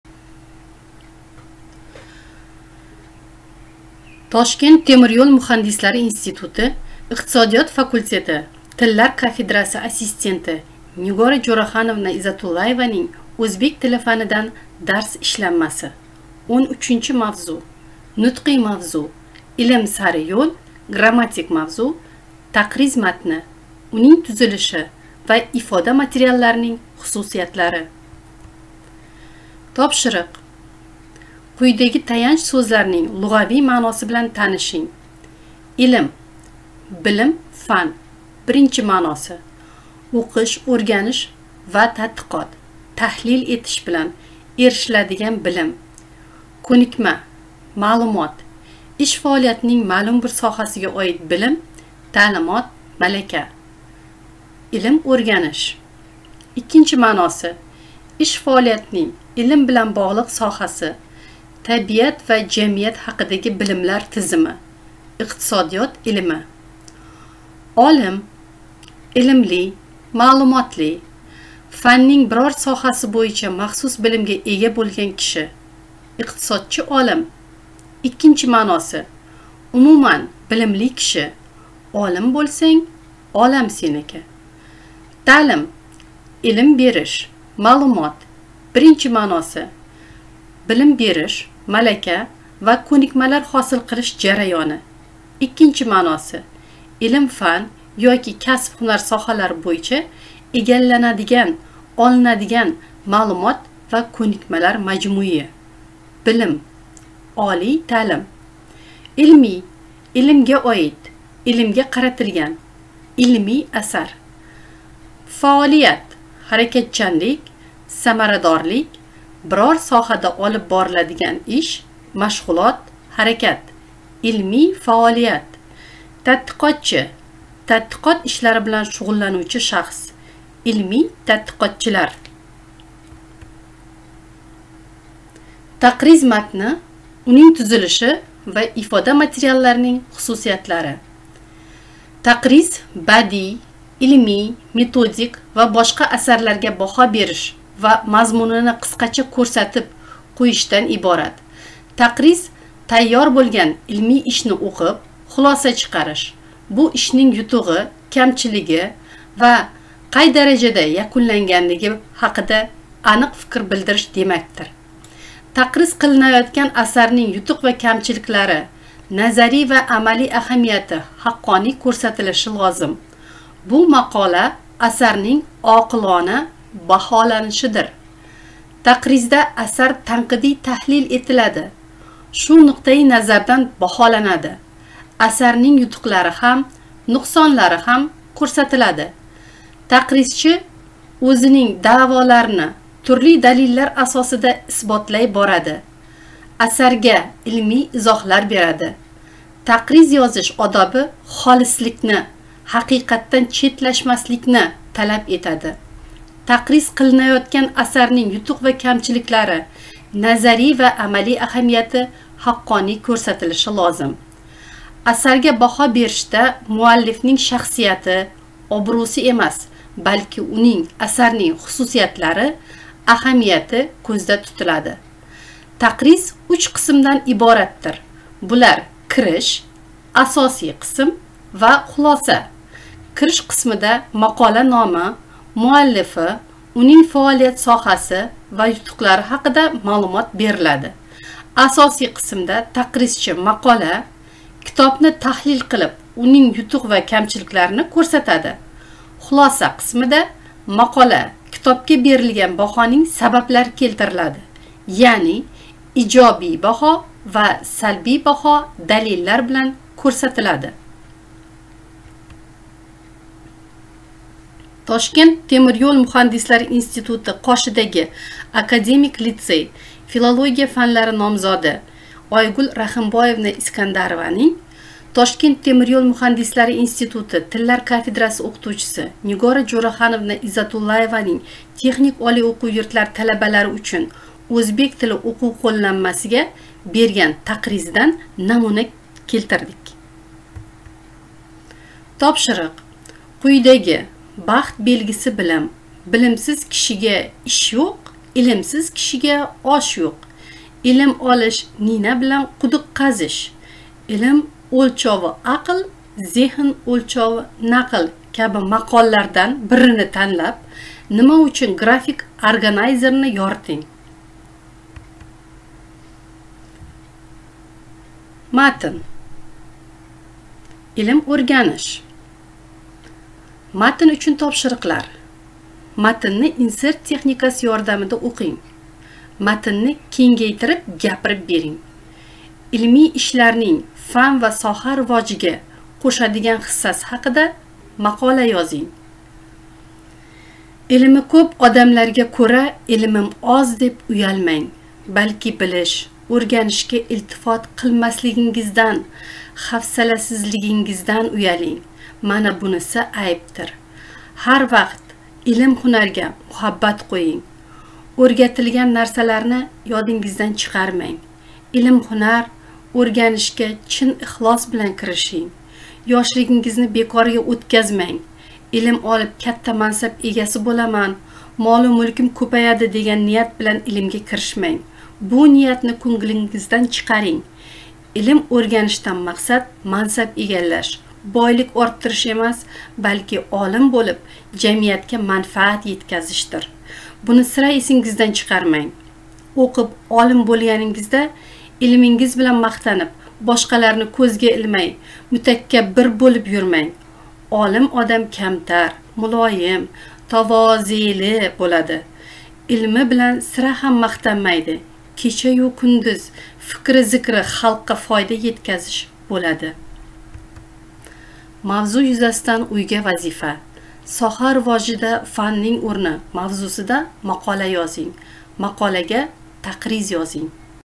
Kurs / O'zbek Tili Fani / 13 Dars / Mavzu: Taqriz Matni, Uning tuzilishi va Ifoda Materallarining...
Toshkent Temir yo'l muhandisligi Instituti talabalari uchun Online video darsligi. kurs talabalari O'zbek tili fanidan

O'qtuvchi: Izzatullayeva N.J.